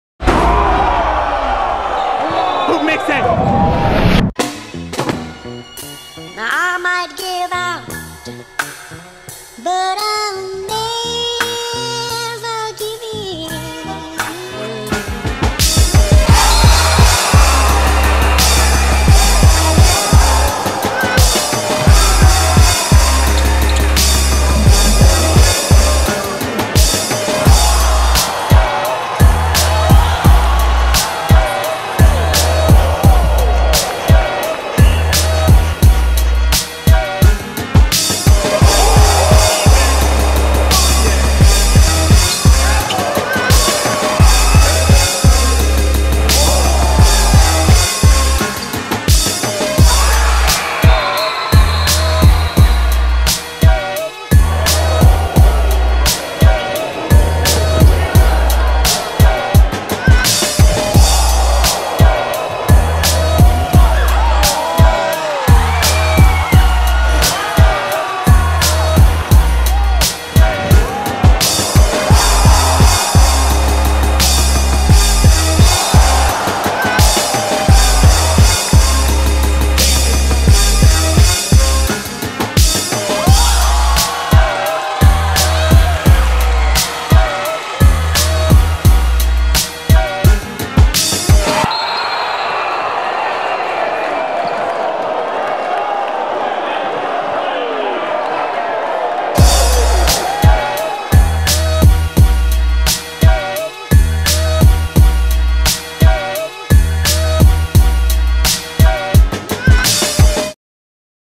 Emmanuel Mudiay The BEST PG In The NBA Draft? OFFICIAL Hoopmixtape
Here is 6'5 Emmanuel Mudiay's Official Hoopmixtape. Watch the NBA Draft tonight to see what team Mudiay ends up on. He is projected to go 1-5 in the first round. Emmanuel Mudiay played his high school basketball at Prime Prep University then played overseas for the Guangdong Southern Tigers of the Chinese Basketball Association. More of Emmanuel Mudiay and other NBA Draft players on Hoopmixtape.com!